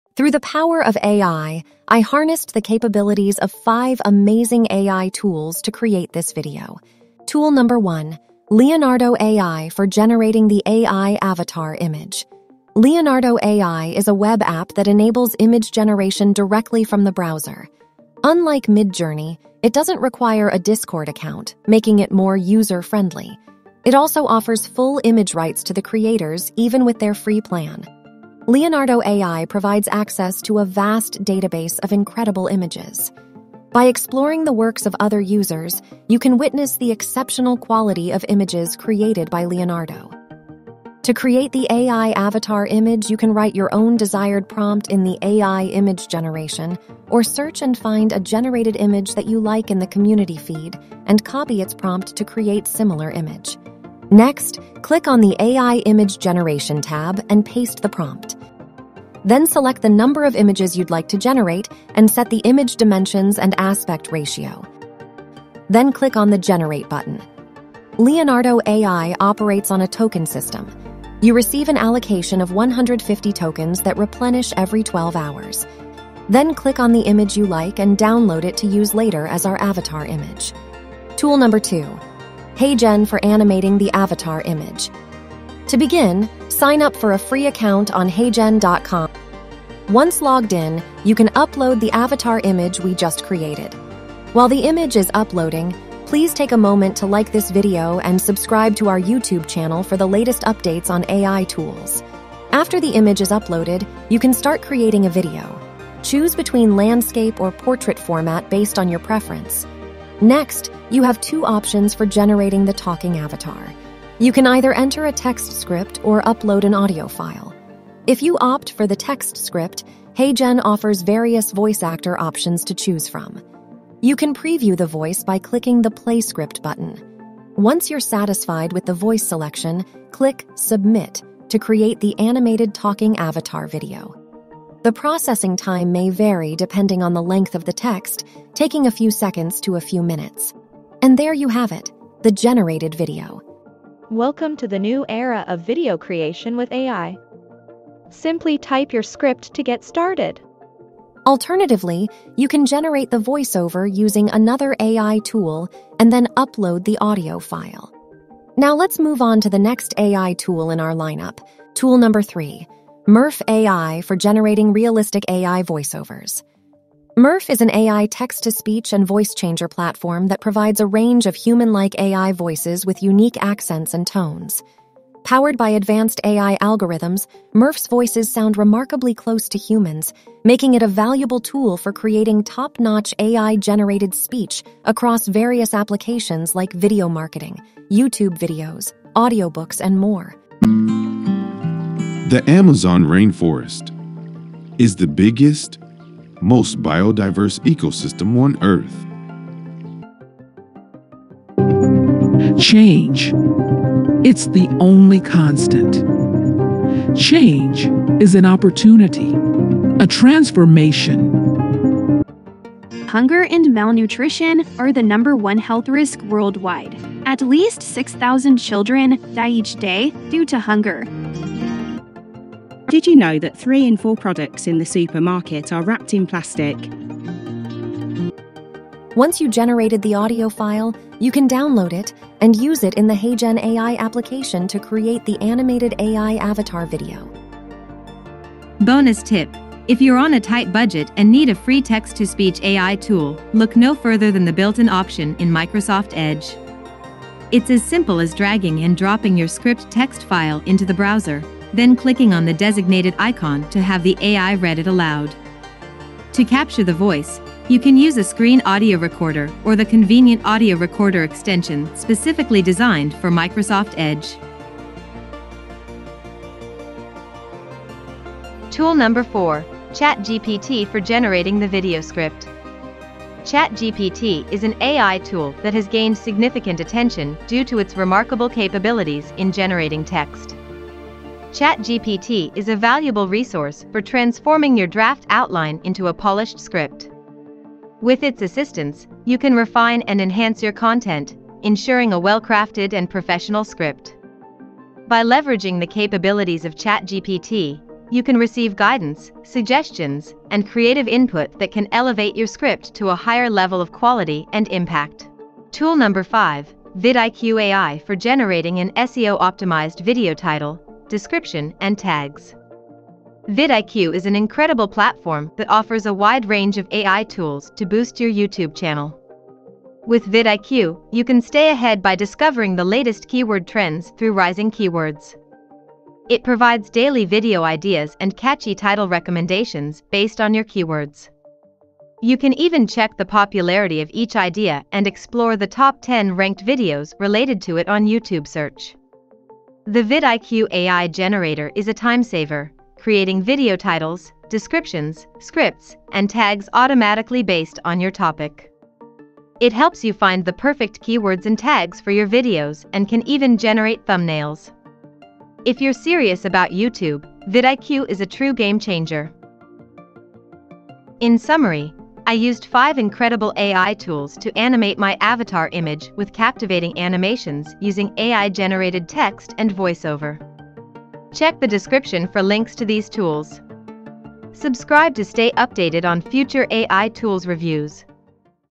5 Revolutionary AI Tools to Create Faceless Videos: Boost Your Content & Income!
I used 5 amazing AI tools to create this video:
Leonardo AI for generating the AI Avatar image,

HeyGen for animating the avatar image,

ChatGPT for converting my draft into a polished video script, and

These cutting-edge AI tools allowed me to generate a mesmerizing avatar image and bring it to life with captivating animations, all driven by AI-generated text and voiceover. Join me as I reveal these remarkable tools and their functionalities, and witness the mind-blowing result they produced.